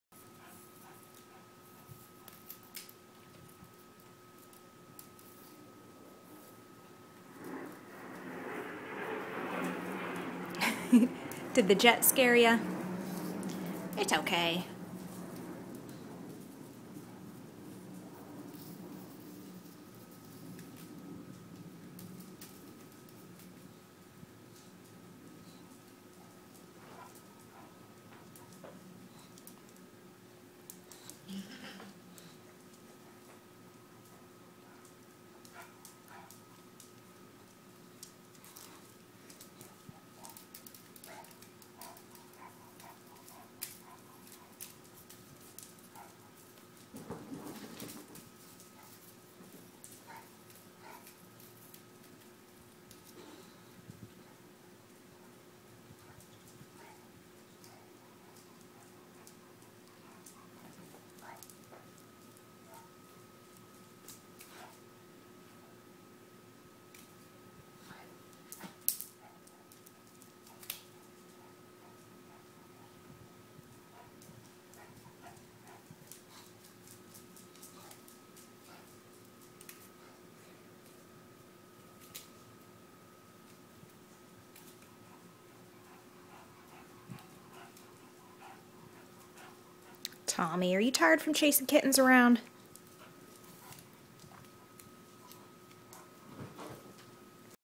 Naked kittens play with Cat Dancer
Camo gets distracted because our English Bulldog keeps chasing her but Lacey loves the Cat Dancer toy!